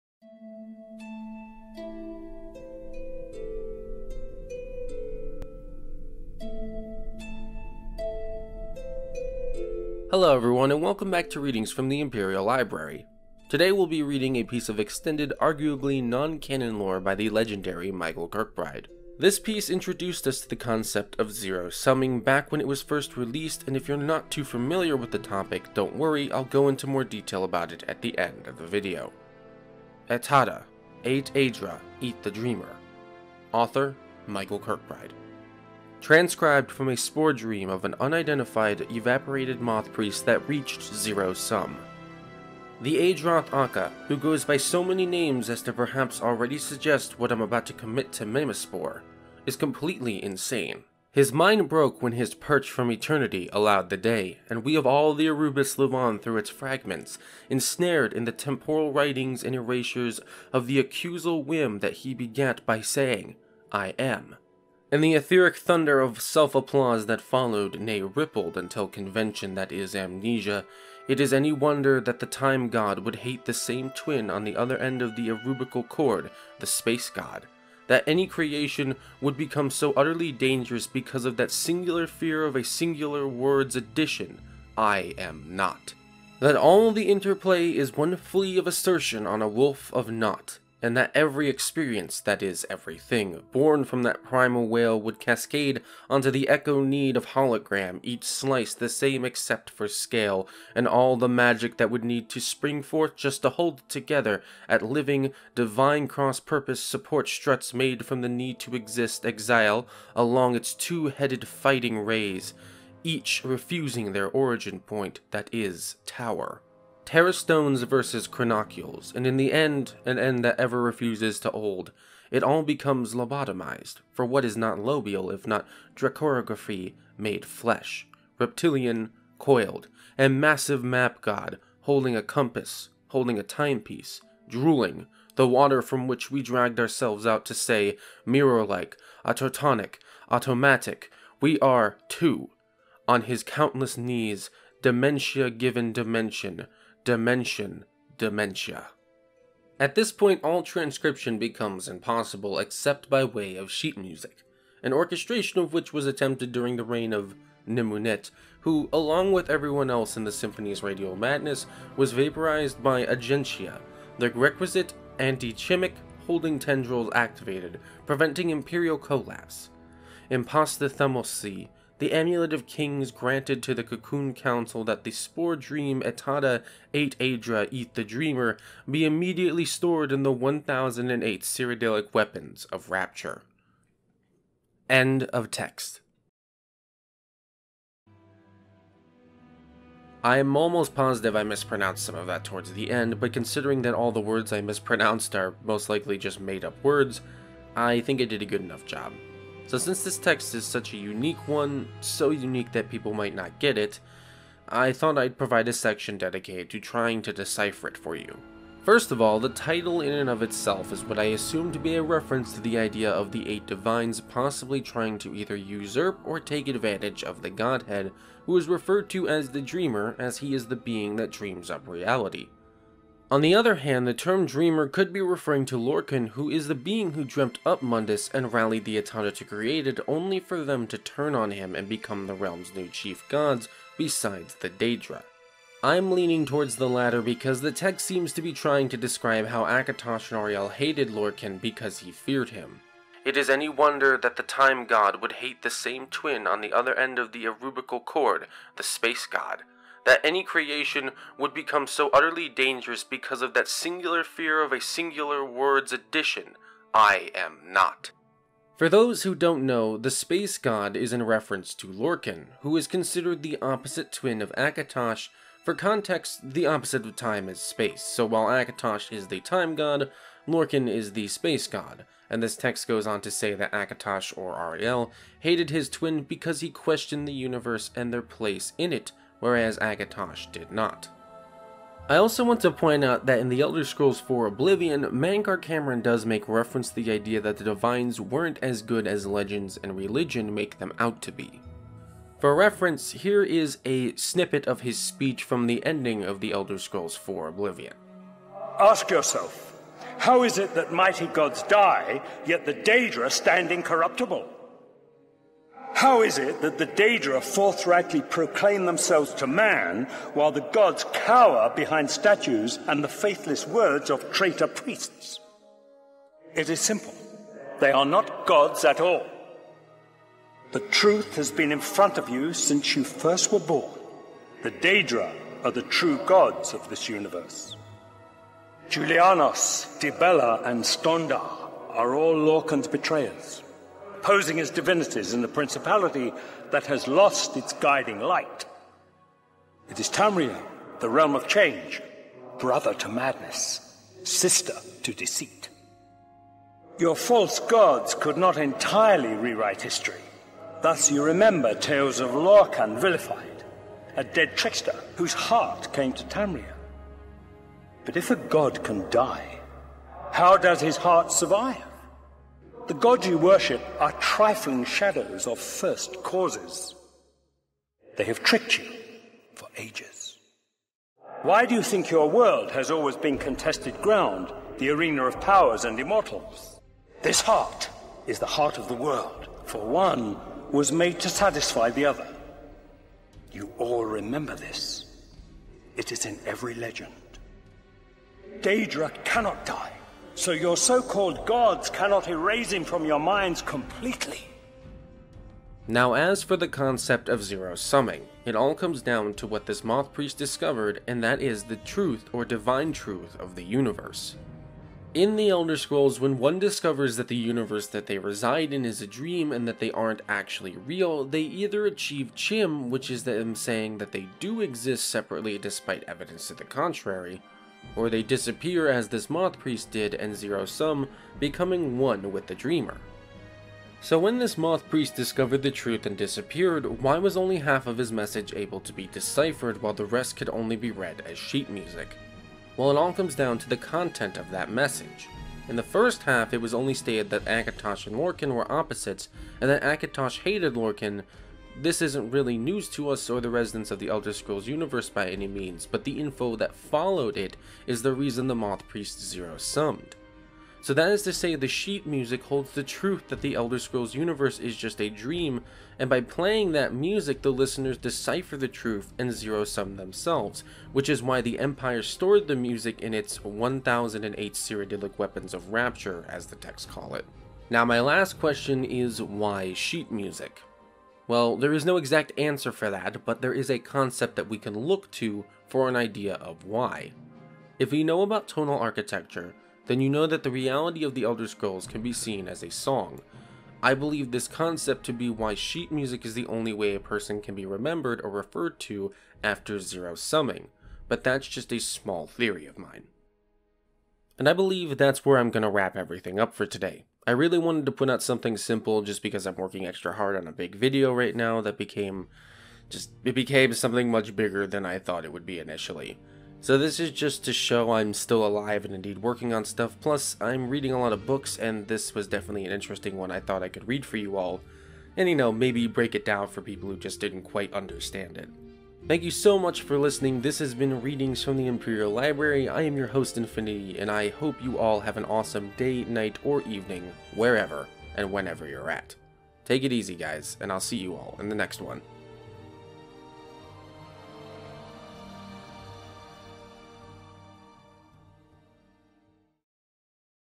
"et'Ada, Eight Aedra, Eat the Dreamer" (Audiobook + Explanation), Readings from The Imperial Library
" That any Creation would become so utterly dangerous because of that singular fear of a singular word's addition: "I AM NOT"? "

Hit me up here! -